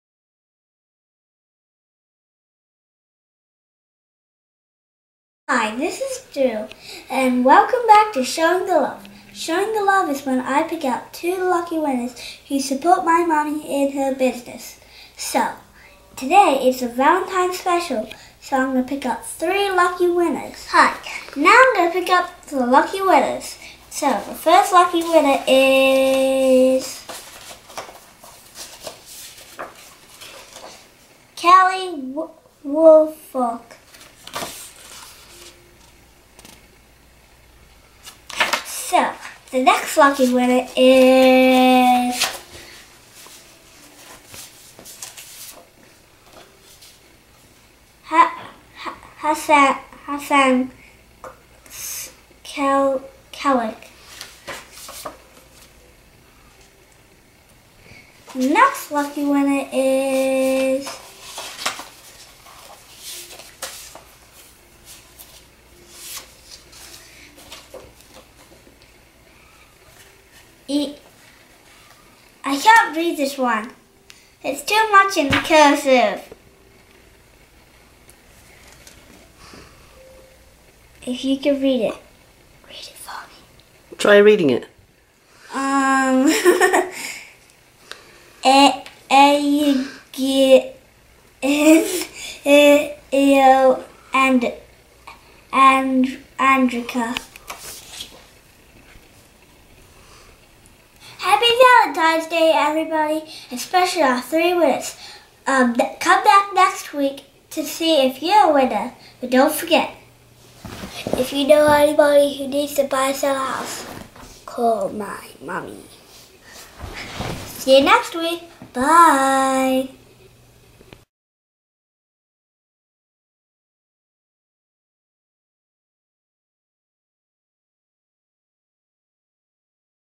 "Showing the Love" McIntire Home Team Client Appreciation in February 2014
Here is the 2nd week of the Annual "Showing the Love" Client Appreciation for my past buyers and sellers in Howard County and Catonsville. This close to Valentines day we have 3 LUCKY winners who will all get a $50 Gift Certificate to Outback Steakhouse. to get more info on Ellie McIntire & The McIntire Home Team of Keller Williams Realty Centre